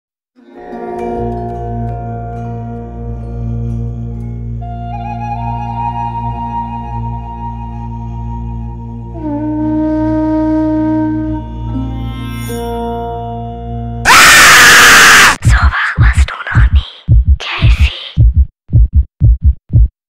K-fee Buddha commercial in HD (German)
K-fee Buddha commercial in crispy 720p. Germany, April 2005.